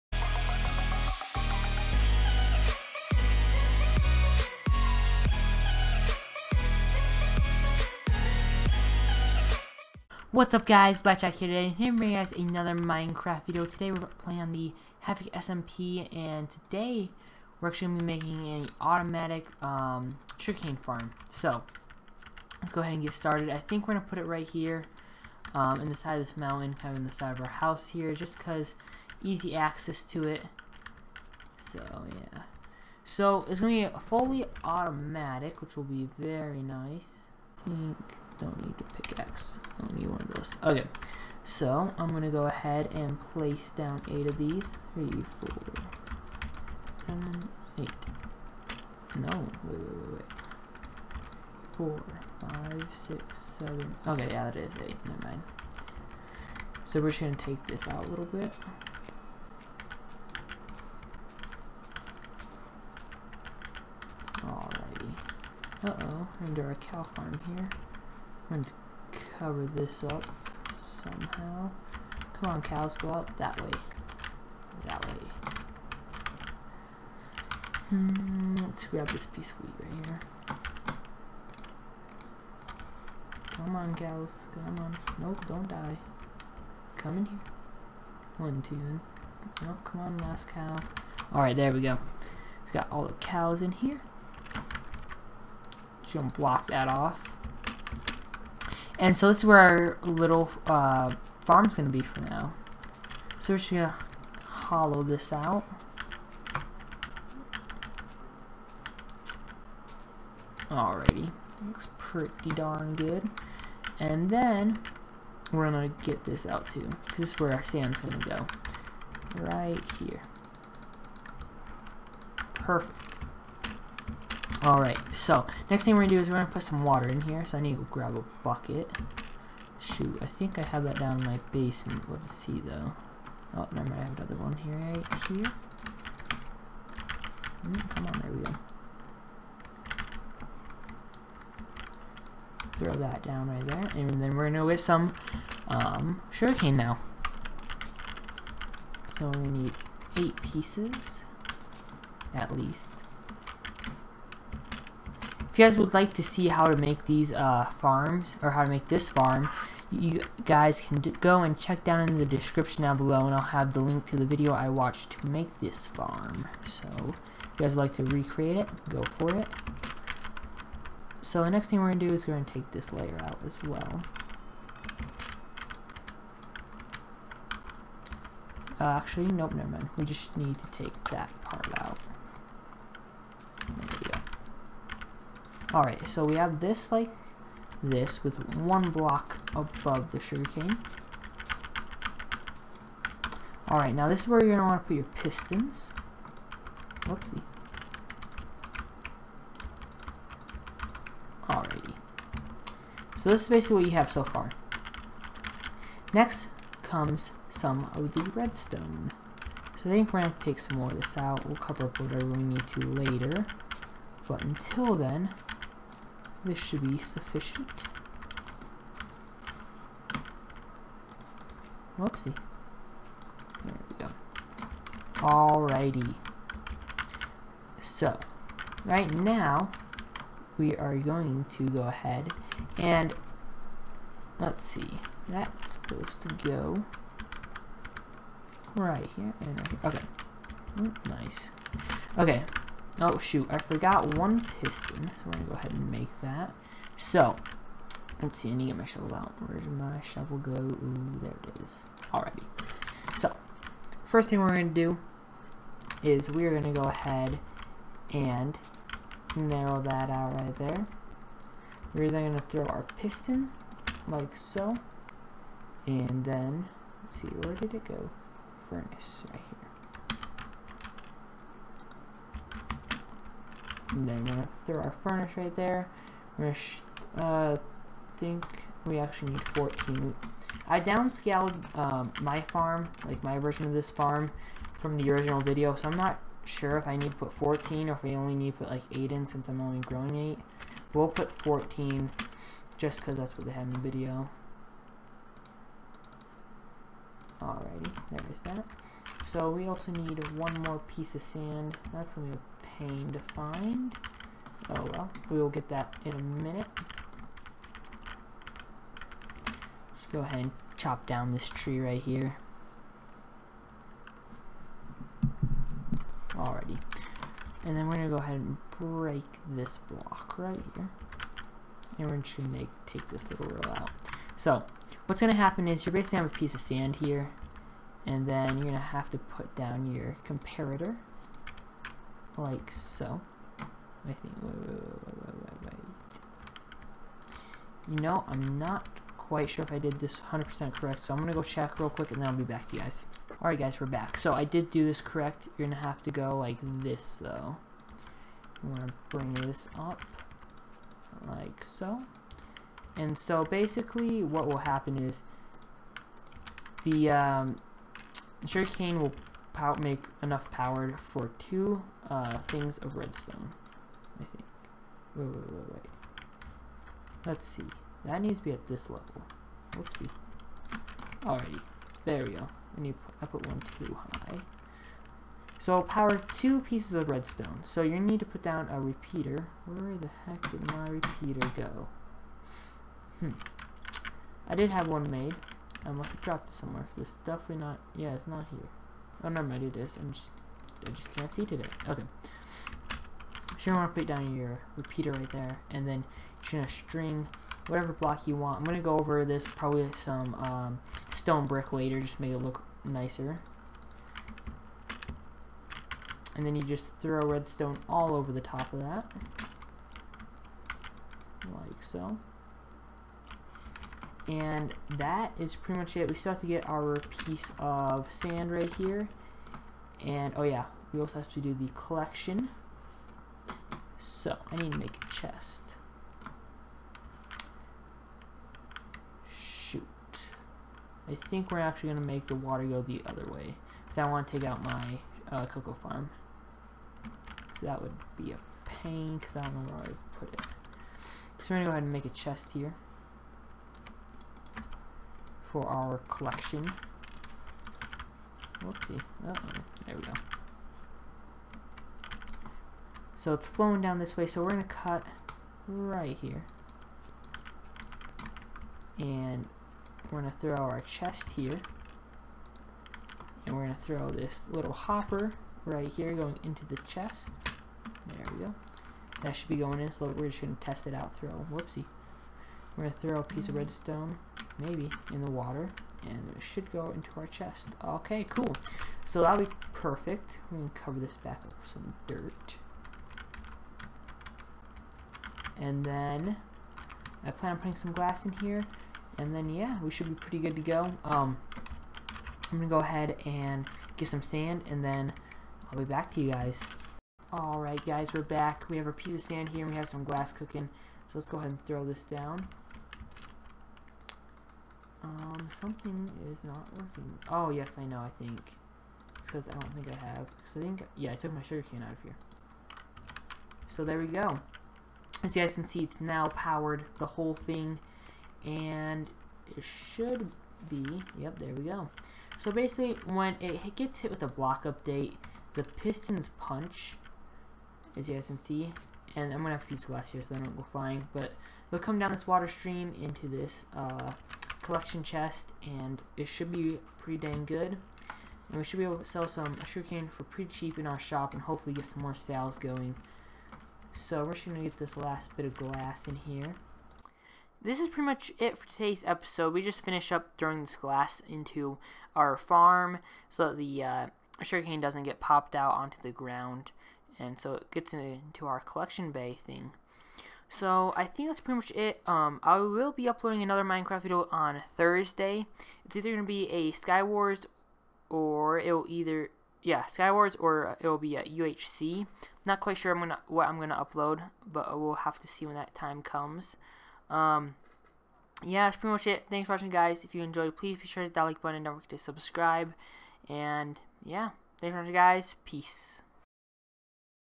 Havoc SMP Building A Automatic Sugar Cane Farm (S:2 E:4)
Today I make an automatic sugar cane farm if you would like to see the original tutorial on how to make the farm it will be the first link down below.